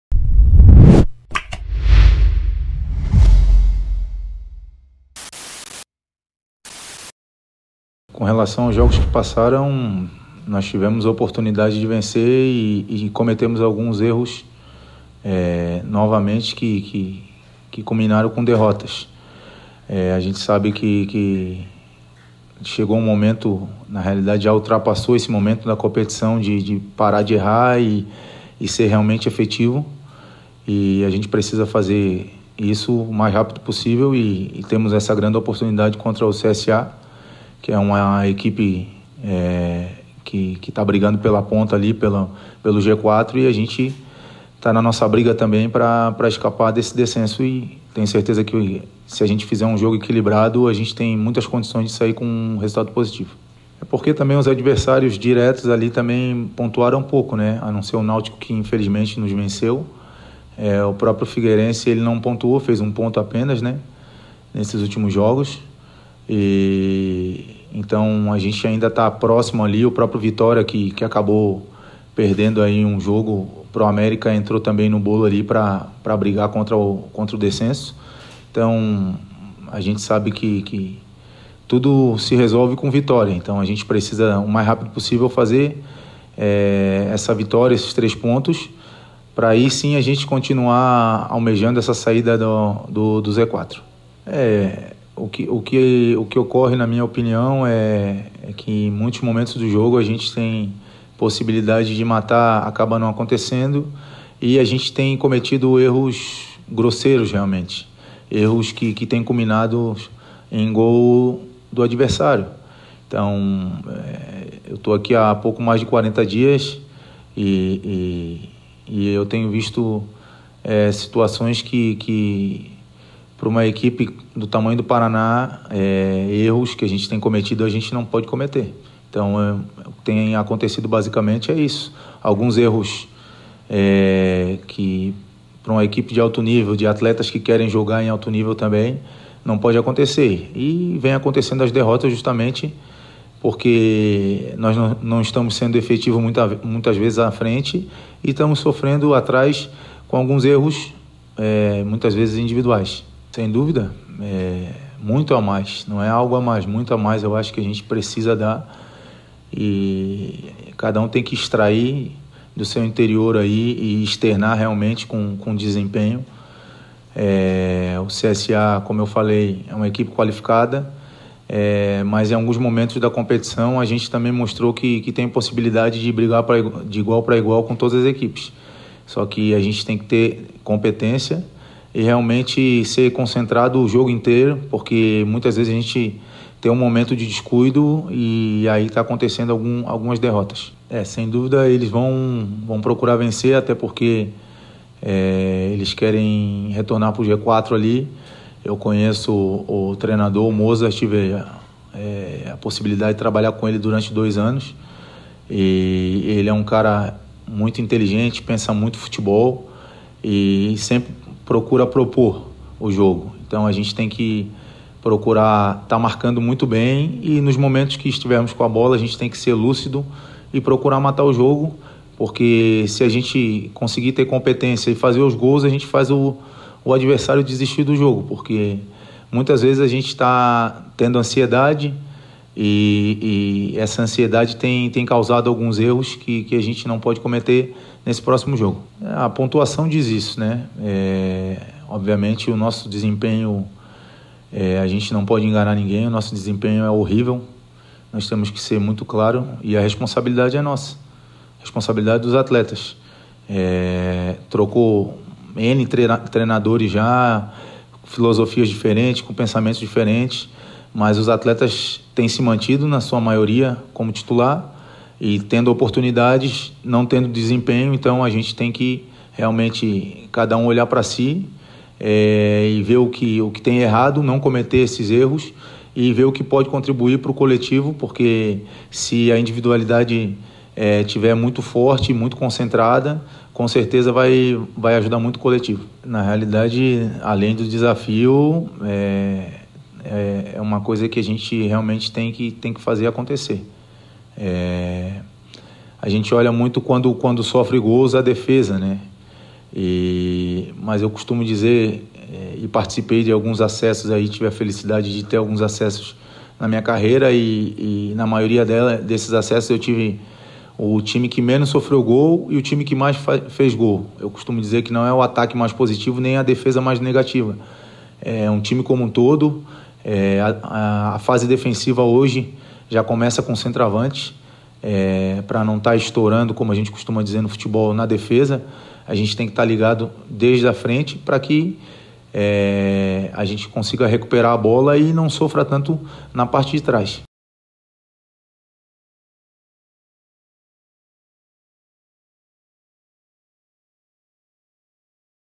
ENTREVISTA COM ZAGUEIRO RAFAEL LIMA
O zagueiro Rafael Lima conversou com a TVPRC e falou sobre os desafios das próximas rodadas e também da importância do próximo jogo, onde o TricolorDaVila enfrenta a equipe do CSA que ainda luta por uma vaga no G4 em jogo válido pela 34ª rodada do CampeonatoBrasileiro da SérieB. Confira!